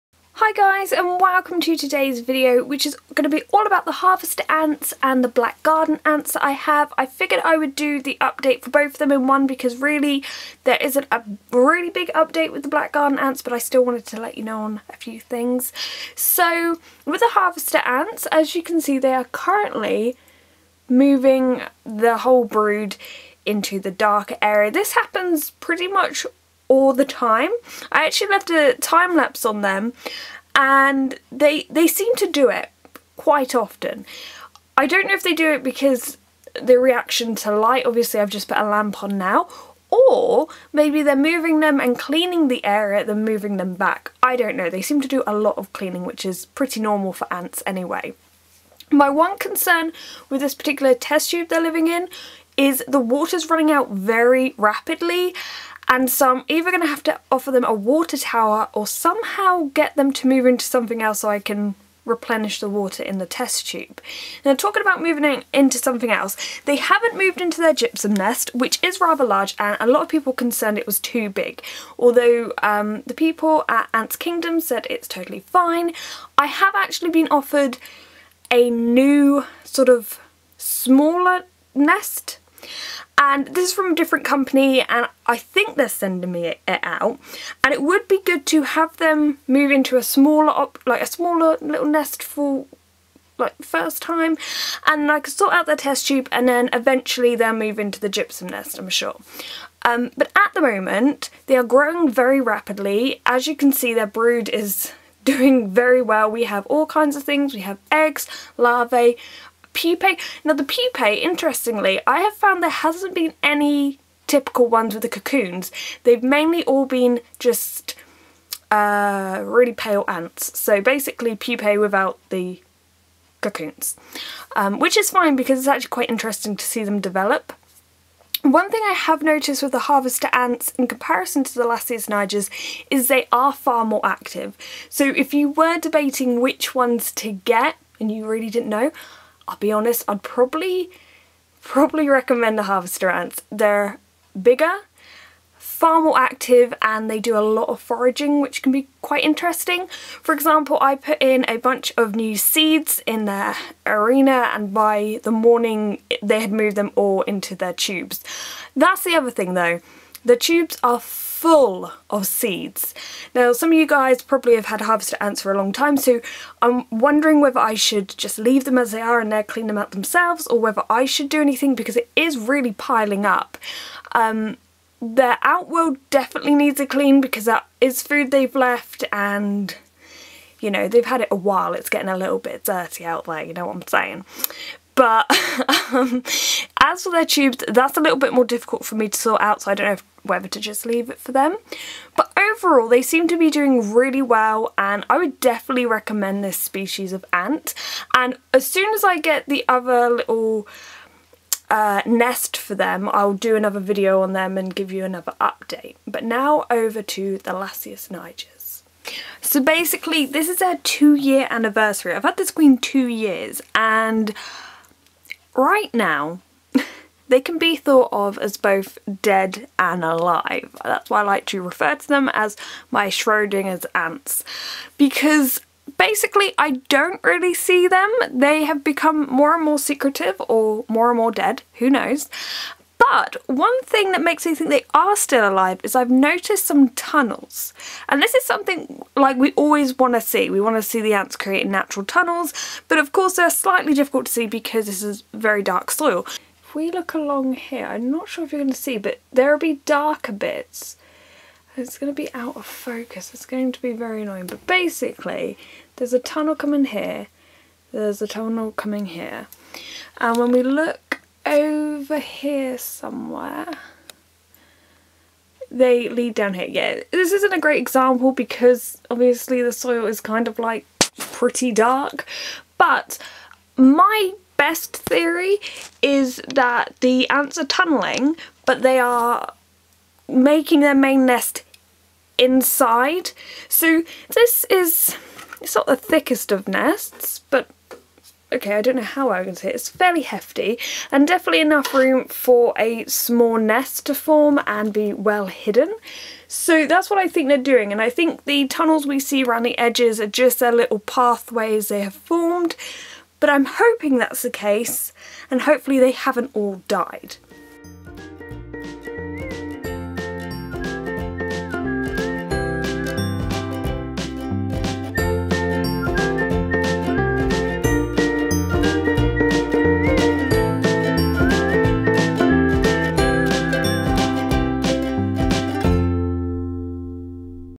TWO SPECIES. ONE UPDATE. Harvester Ants vs Black Garden Ants.
In today's video my Lasius Niger Queen finally turns 2! And I update you on how my two species of ants are getting on - one being in a natural setup, the other being in a traditional test tube and outworld setup.